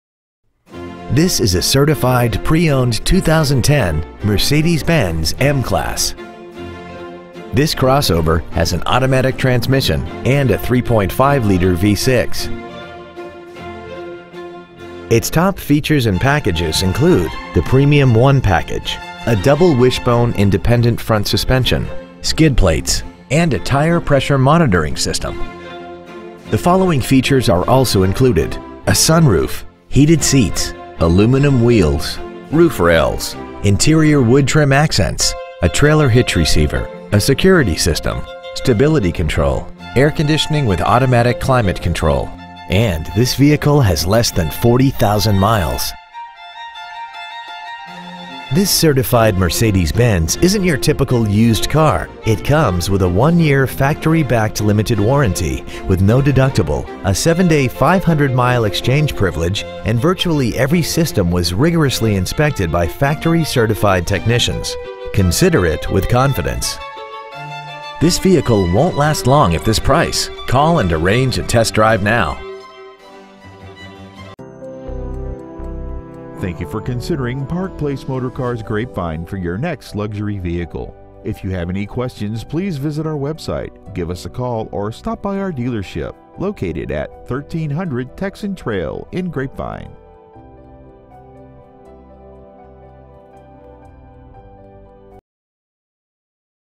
Certified 2010 Mercedes-Benz M Class Dallas TX
Year : 2010
 Make : Mercedes-Benz
 Model : M Class
 Engine : 3.5L DOHC 24-valve V6 engine
 Trans . : Automatic
 Exterior : Gray
 Miles : 39,521
 Interior : Black
 Stock : MC117208